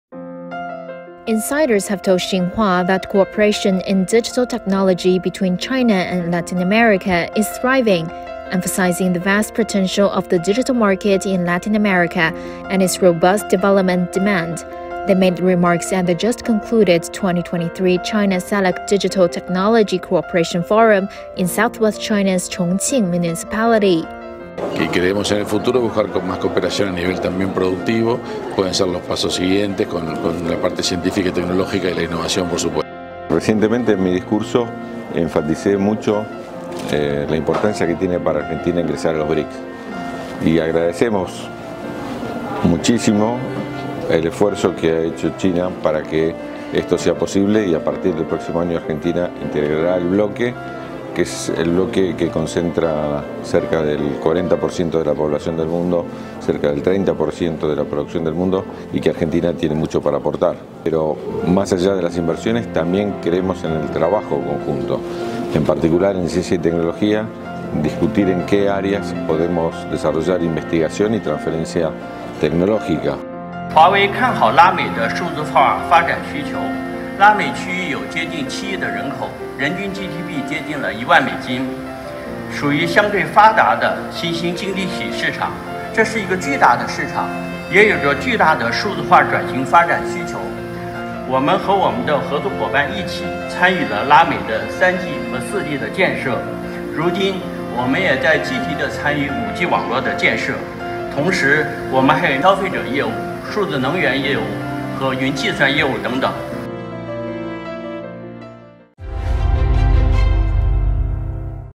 GLOBALink | China, LatAm deepen digital cooperation, seek new opportunities for development
Participants at a forum have said that cooperation in digital technology between China and Latin America is thriving, emphasizing the vast potential of the digital market in Latin America and its robust development demand.